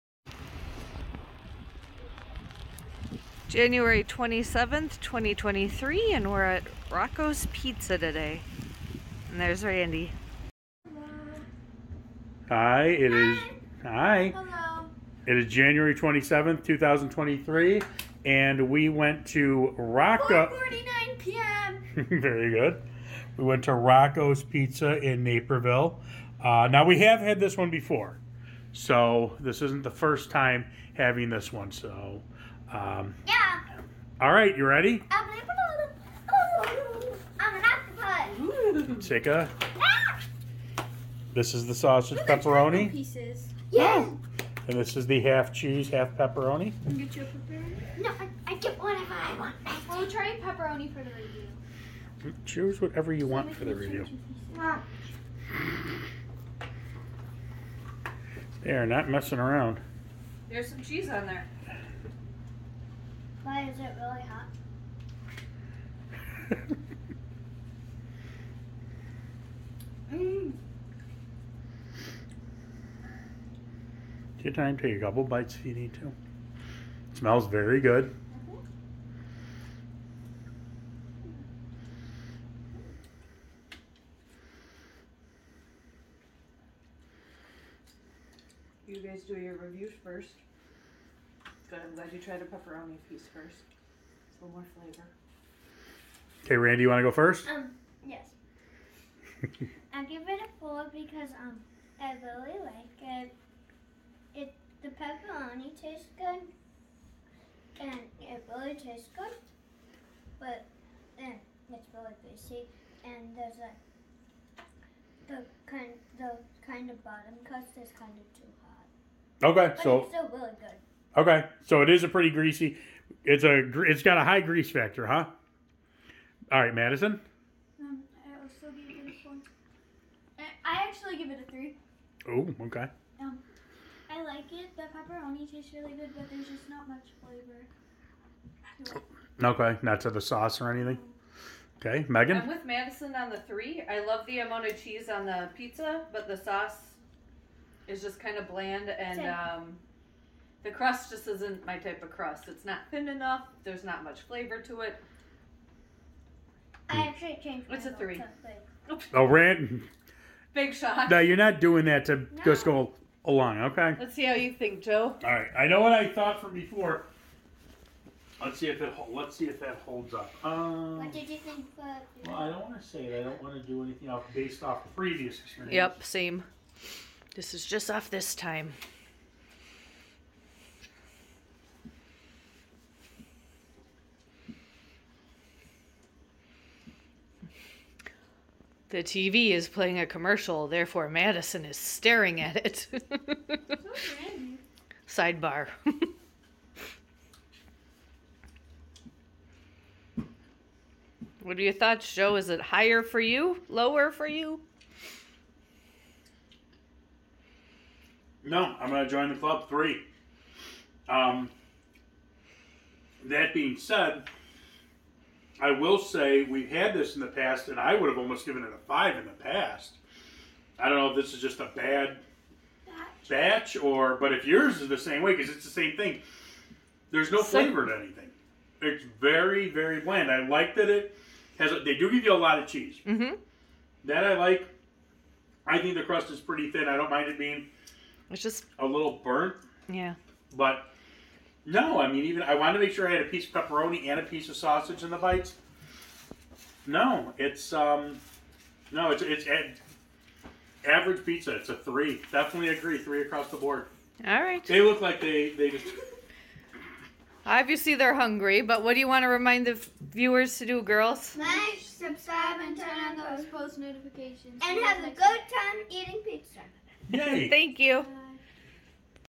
Review of Rocco's Pizza Naperville, IL - Chicago Pizza Review
We go to Rocco's Pizza in Naperville, IL
192 W Gartner Rd #168, Naperville, IL 60540